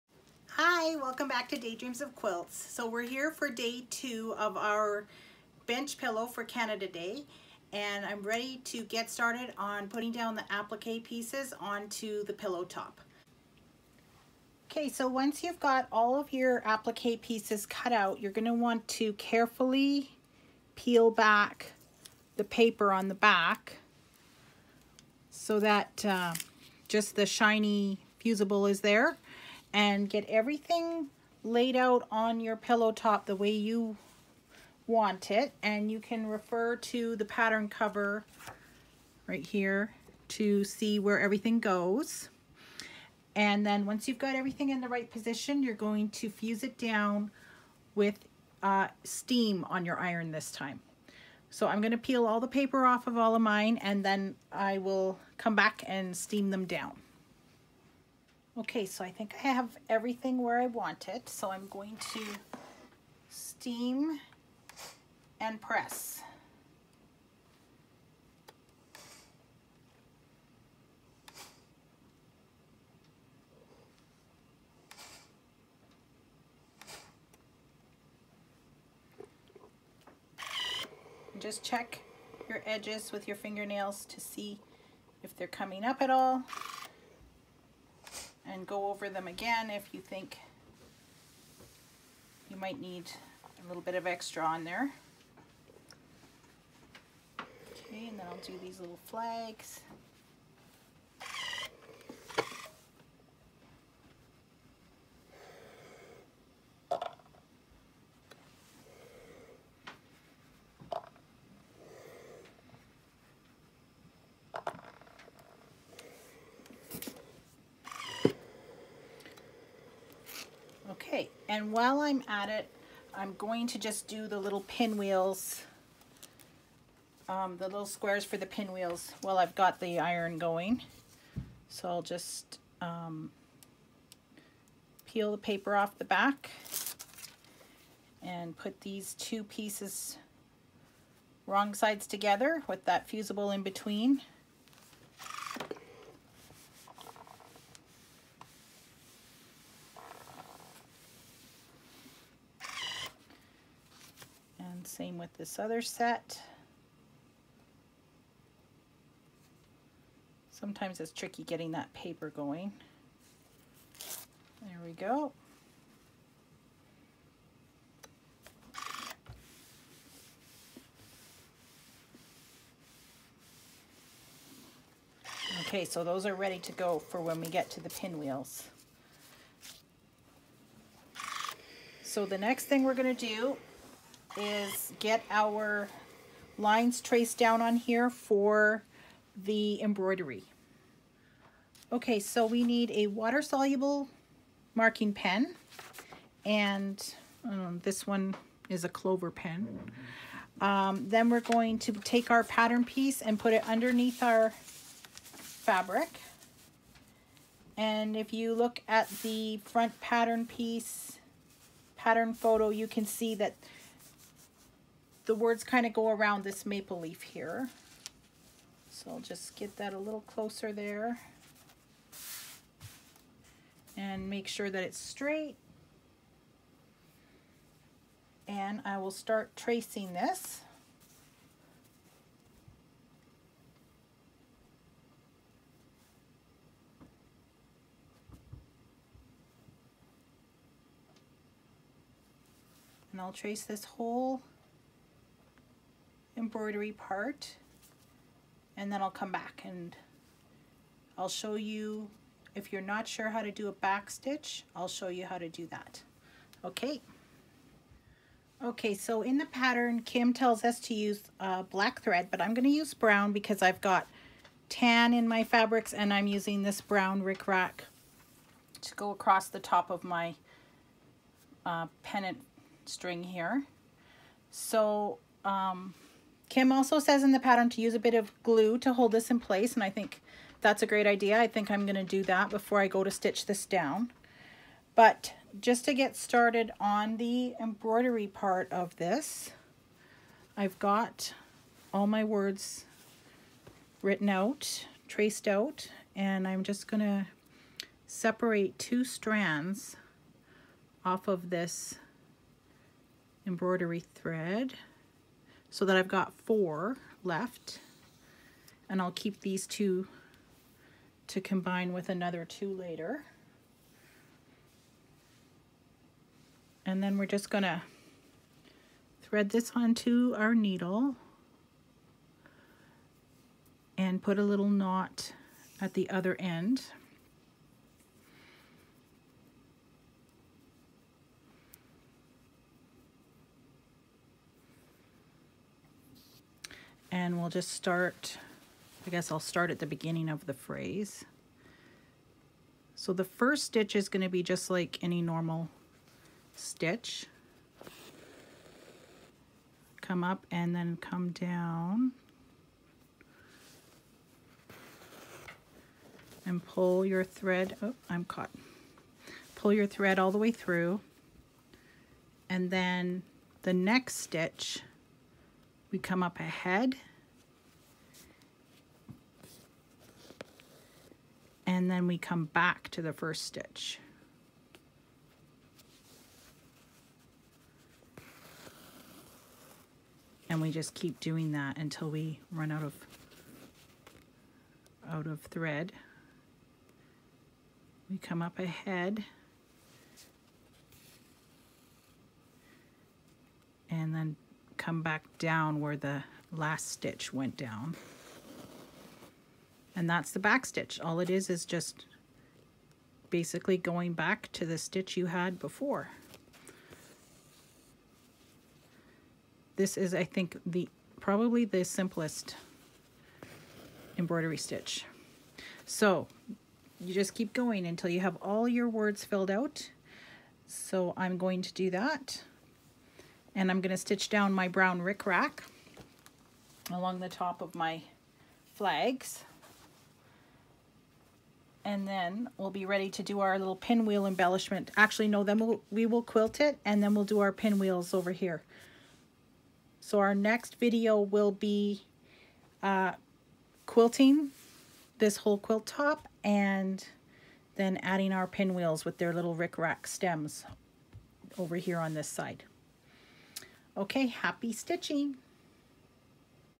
Making a Canada Day Bench Pillow Day 2
Making a large Canada Day pillow to celebrate Canada Day 2020 at home using the Kimberbell pattern “O Canada”.